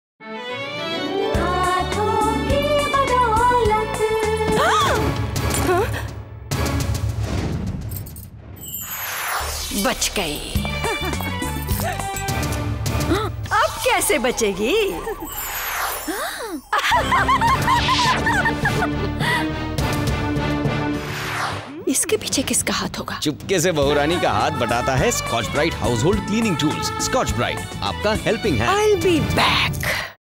Scotch Brite- Range
This is the opening episode of the serial. It establishes the characters.

CLIENT: 3M
AGENCY: GREY WORLDWIDE
CREATIVE: VISHNU & SHYAM
DIRECTOR: PUSHPENDRA MISRA
PRODUCTION HOUSE: FLYING SAUCER